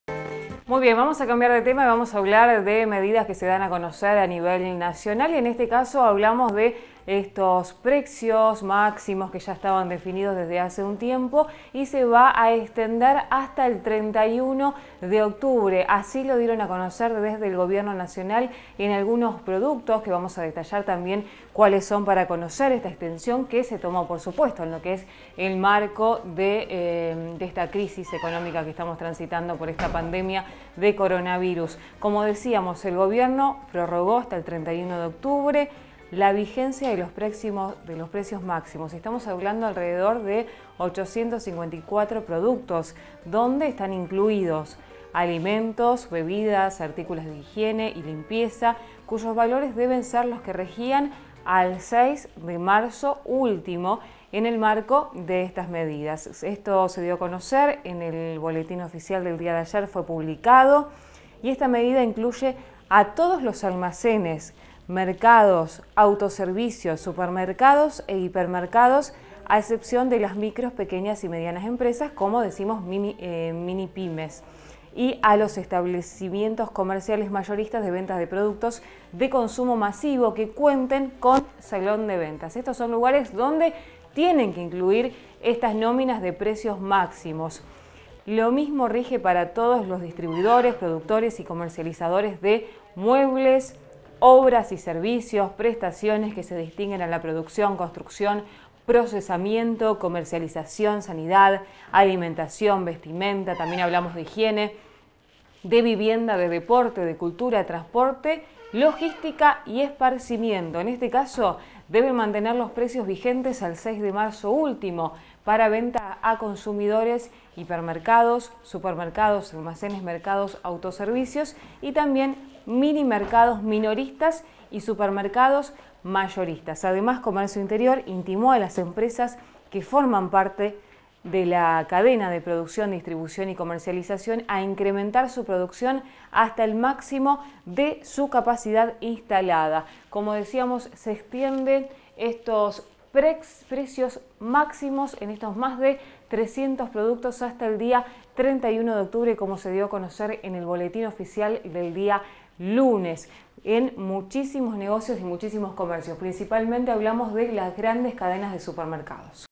Extienden los plazos para el programa Precios Máximos
Las autoridades gubernamentales han resuelto la extensión hasta fines de octubre del programa denominado Precios Máximos.